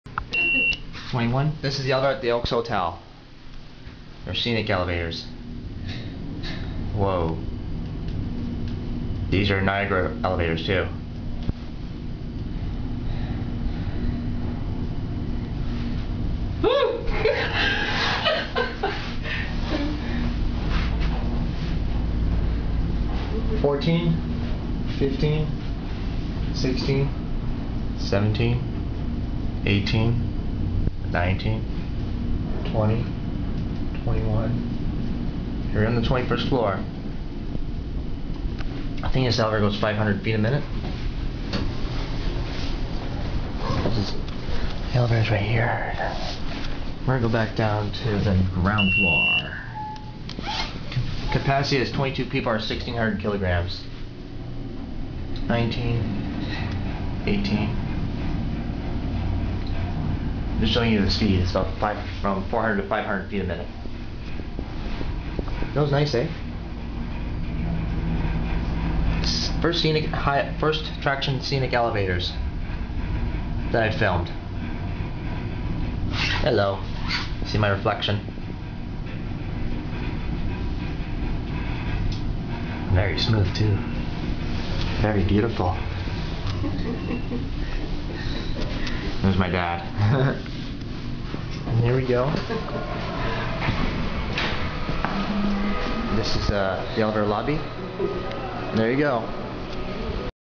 Niagara Scenic Traction Elevator @ the Oakes Hotel Niagara Falls
Riding the very nice scenic elevator at the Oakes Hotel in Niagara Falls.

Tech. Specs:

General: Niagara/Geared Traction

Capacity: 22 people/1600kg/3500lbs

Speed & Dist: 21 floors/215ft/400FPM

Power: 10HP

Door Safety: Full Sensor

Year Installed: ???